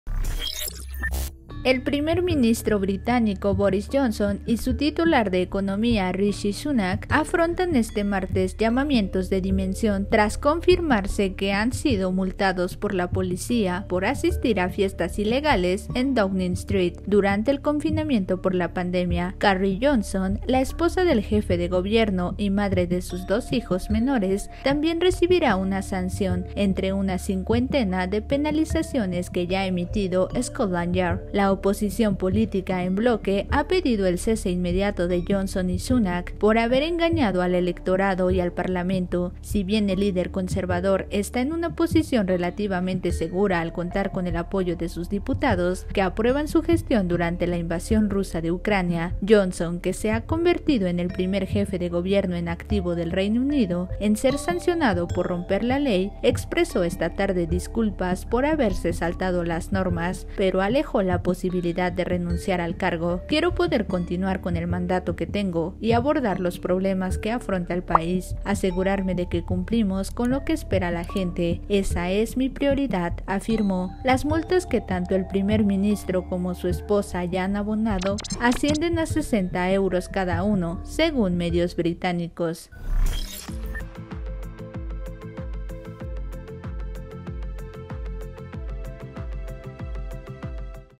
Multan a Boris Johnson y a su ministro de Economía por "Partygate" en Reino Unido
Carrie Johnson, la esposa del jefe del Gobierno y madre de sus dos hijos menores, también recibirá una sanción, por violar las normas durante el confinamiento por la epidemia de Covid